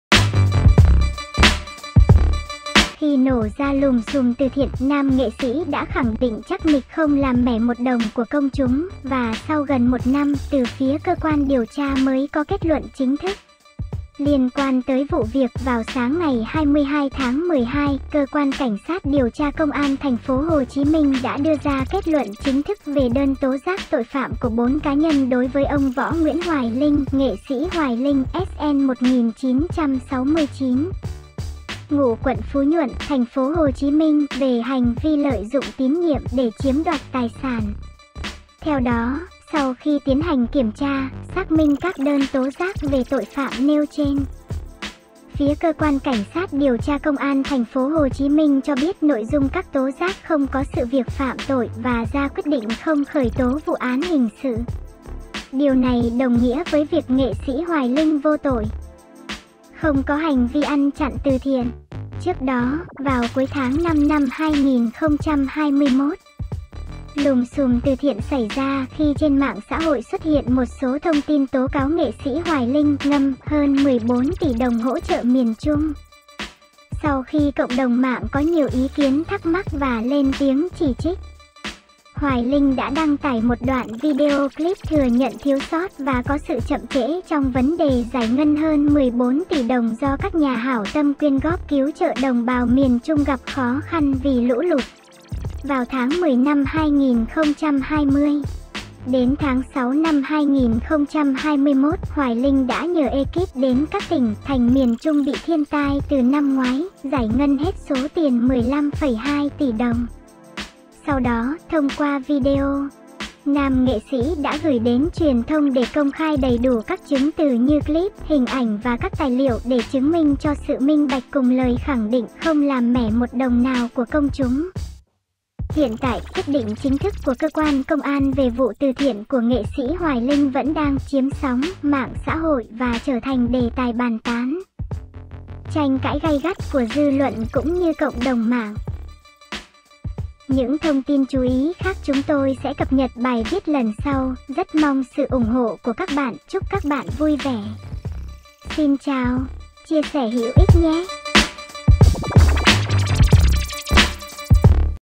Nghệ sĩ Hoài Linh không ăn chặn tiền từ thiện
Khi nổ ra lùm xùm từ thiện, nam nghệ sĩ đã khẳng định chắc nịch “không làm mẻ một đồng của công chúng”, và sau gần 1 năm, từ phía cơ quan điều tra mới có kết luận chính thức.

Liên quan tới vụ việc, vào sáng ngày 22/12, Cơ quan CSĐT Công an TP.HCM đã đưa ra kết luận chính thức về đơn tố giác tội phạm của 4 cá nhân đối với ông Võ Nguyễn Hoài Linh (nghệ sĩ Hoài Linh, SN 1969, ngụ quận Phú Nhuận, TP.HCM) về hành vi lợi dụng tín nhiệm để chiếm đoạt tài sản.

Theo đó, sau khi tiến hành kiểm tra, xác minh các đơn tố giác về tội phạm nêu trên, phía Cơ quan CSĐT Công an TP.HCM cho biết "nội dung các tố giác không có sự việc phạm tội" và ra quyết định không khởi tố vụ án hình sự. Điều này đồng nghĩa với việc nghệ sĩ Hoài Linh vô tội, không có hành vi ăn chặn từ thiện.

Trước đó, vào cuối tháng 5/2021, lùm xùm từ thiện xảy ra khi trên mạng xã hội xuất hiện một số thông tin tố cáo nghệ sĩ Hoài Linh "ngâm" hơn 14 tỷ đồng hỗ trợ miền Trung. Sau khi cộng đồng mạng có nhiều ý kiến thắc mắc và lên tiếng chỉ trích, Hoài Linh đã đăng tải một đoạn Video Clip thừa nhận thiếu sót và có sự chậm trễ trong vấn đề giải ngân hơn 14 tỷ đồng do các nhà hảo tâm quyên góp cứu trợ đồng bào miền Trung gặp khó khăn vì lũ lụt vào tháng 10/2020.

Đến tháng 6/2021, Hoài Linh đã nhờ ê-kíp đến các tỉnh, thành miền Trung bị thiên tai từ năm ngoái, giải ngân hết số tiền 15,2 tỷ đồng.

Sau đó, thông qua video, nam nghệ sĩ đã gửi đến truyền thông để công khai đầy đủ các chứng từ như clip, hình ảnh và các tài liệu để chứng minh cho sự minh bạch cùng lời khẳng định "Không làm mẻ một đồng nào của công chúng".

Hiện tại, quyết định chính thức của cơ quan công an về vụ từ thiện của nghệ sĩ Hoài Linh vẫn đang ‘chiếm sóng’ mạng xã hội và trở thành đề tài bàn tán, tranh cãi gay gắt của dư luận cũng như cộng đồng mạng.